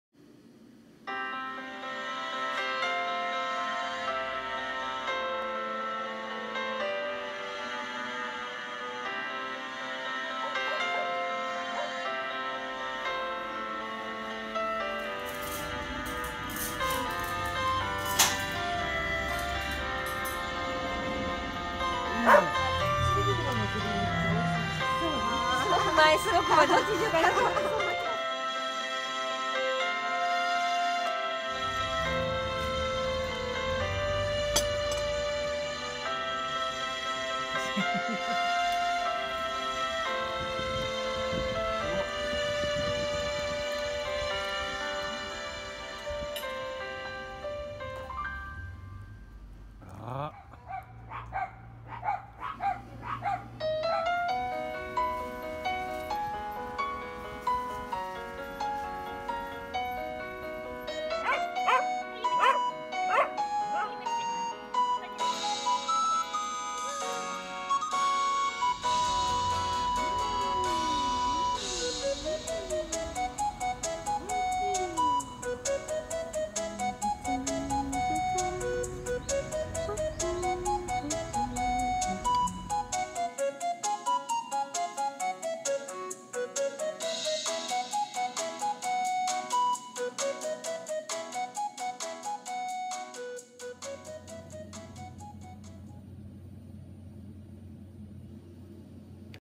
[ドッグカフェ]藤沢市長後のcafe KO-BAに行って来た。
cafe KO-BA名前がなんか 工場みたいですが、いぜんは、ラーメンの製麺所の工場のあとに作られたお店だそうです。
だから、KO-BAなんですね〜
ばしょは、国道476号線 長後駅入口交差点側です。
住所： 神奈川県藤沢市高倉731
カントリー調のお店と併設のドッグランがなかなかいいかんじです。
駐車場は、４台止められスペースがありました。